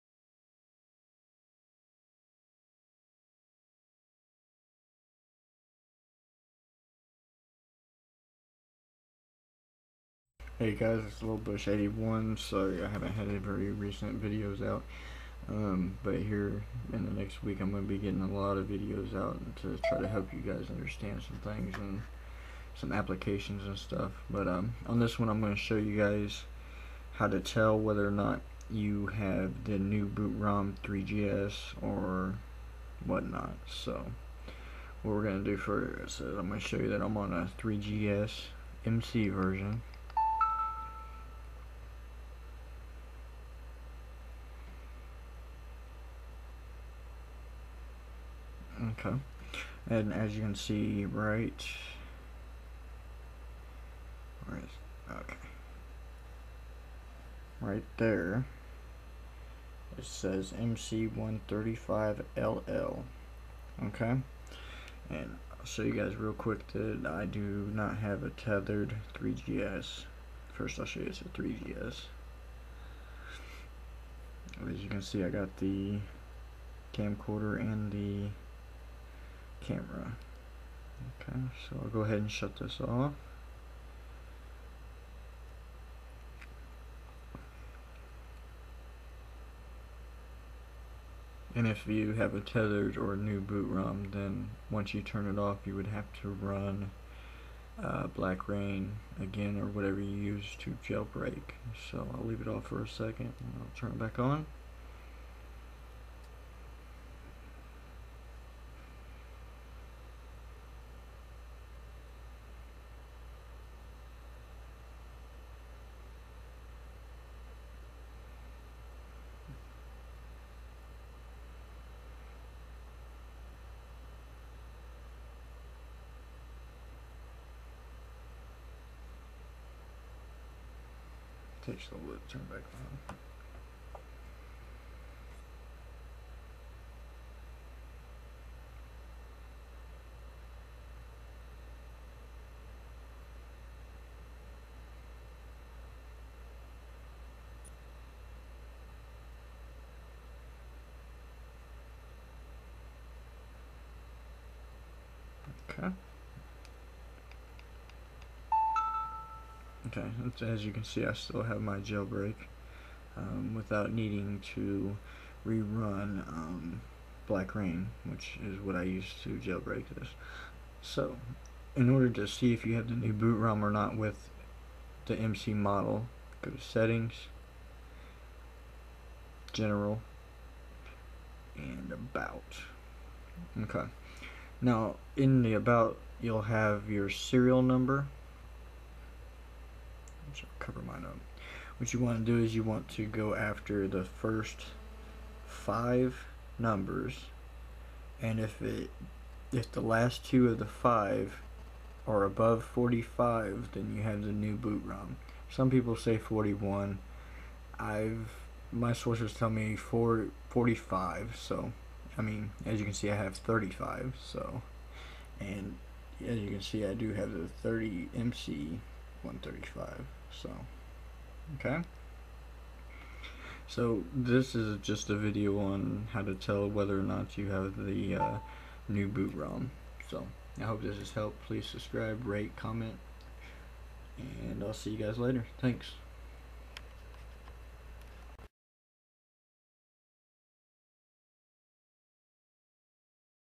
MC New BootRom or Not????
THis is to show you Guys with MC models weather or not you have the new Boot Roms or not :)